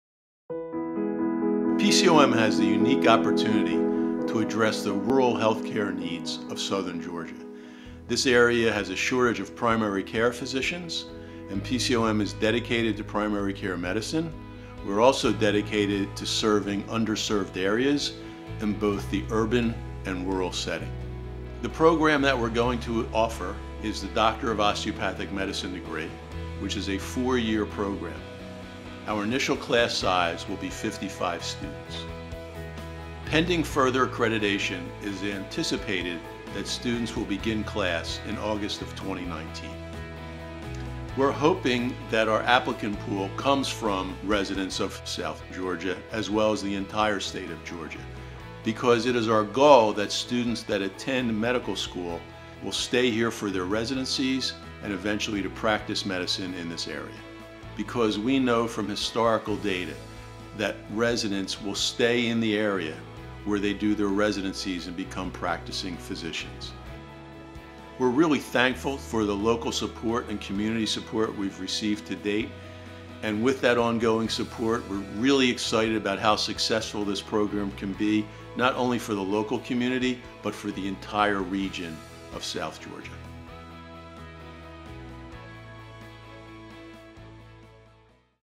Doctor of Osteopathic (DO) | PCOM South Georgia
PCOM has the unique opportunity to address the rural health care needs of the South Georgia region. As a Doctor of Osteopathic (DO) Medicine student at PCOM South Georgia, you’ll learn to treat the person, not just the disease. With a combination of intense training, hands-on learning, interactive courses, and an active faculty, you'll find all the tools and skills needed to diagnose and treat patients.